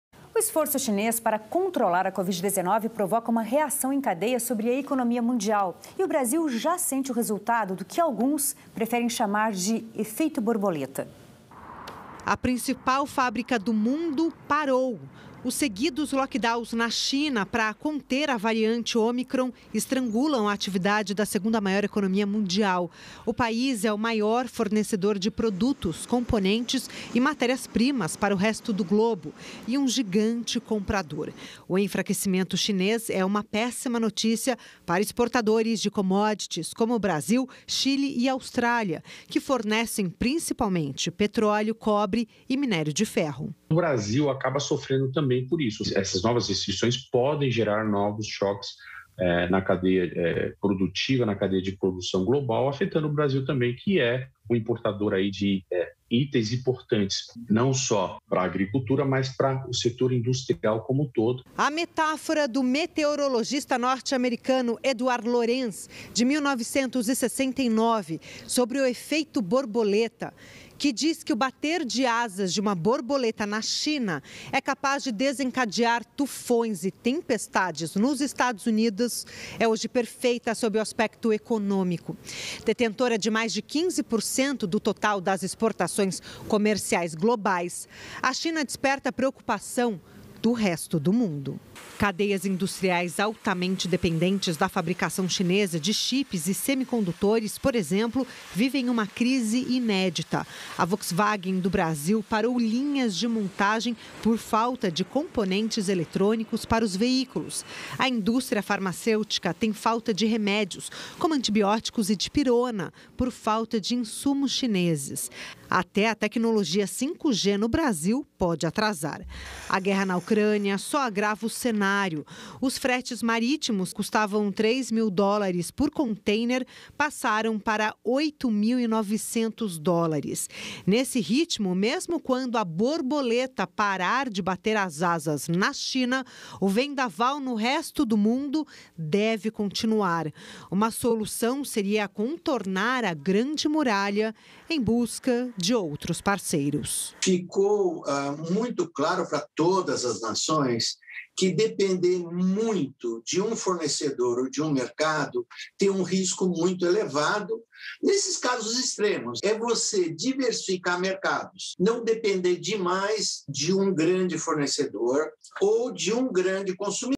Esforço da China para controlar a Covid-19 provoca reação na economia mundial
A principal fábrica do mundo parou. Os seguidos lockdowns na China, para conter a variante ômicron, estrangulam a atividade da segunda maior economia mundial.  O país é o maior fornecedor de produtos, componentes e matérias-primas para o mundo.

Joelson Sampaio, professor de economia da FGV-SP, disse: “O Brasil acaba sofrendo também por isso e essas novas restrições podem gerar novos choques na cadeia produtiva, na cadeia de produção global, afetando o Brasil que é um importador de itens importantes, não só para agricultura mas para os setores industriais como um todo”.